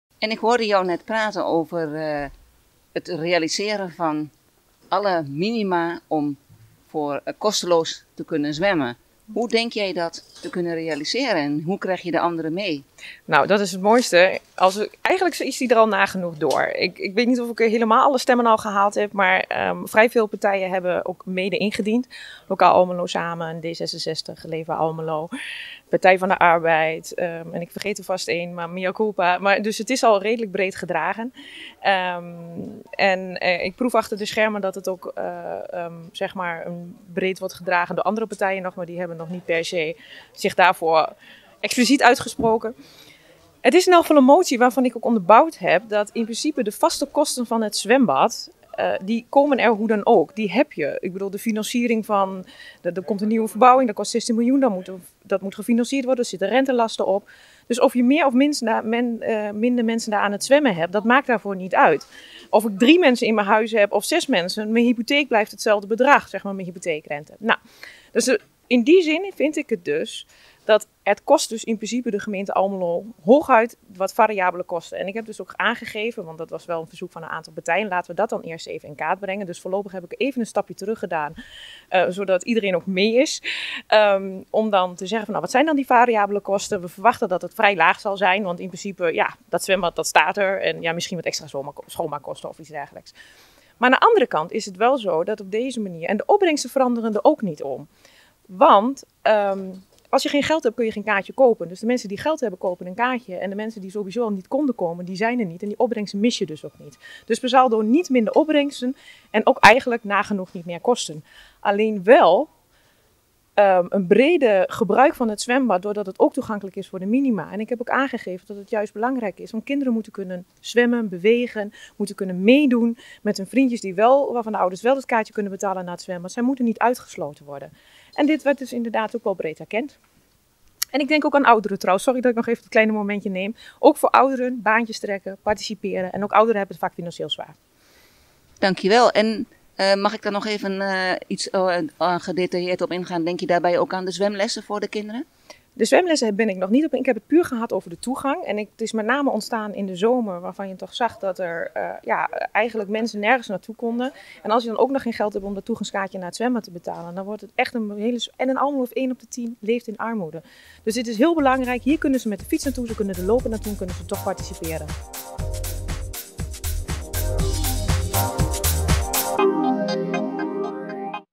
Forum voor rechts wil gratis zwemmen voor minima
Tijdens de behandeling van de gemeentebegroting 2021 pleit Forum voor Rechts, Patricia Pol voor gratis zwemmen voor minima.